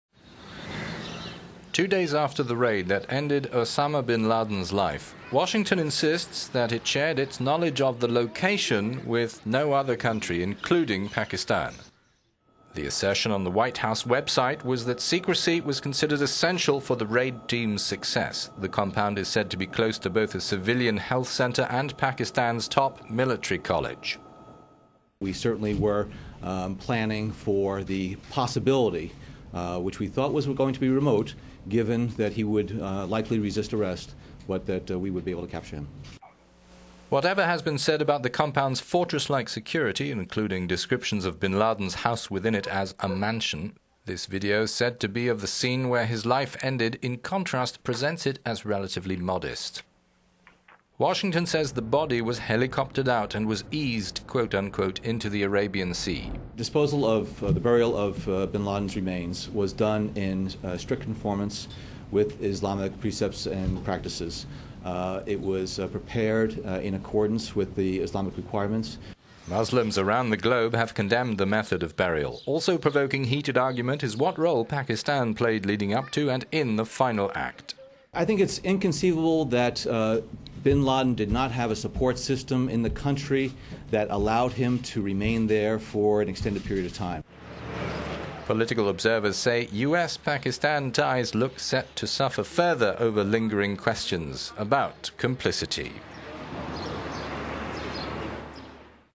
Bin Laden raid strains US-Pakistan ties
Two days after the raid that ended Osama Bin Laden's life, Washington insists that it shared its knowledge of the location with no other country, including Pakistan.

The assertion on the White House website was that secrecy was considered as essential to the raid team's success. The compound is said to be close to both a civilian health centre and Pakistan's top military college.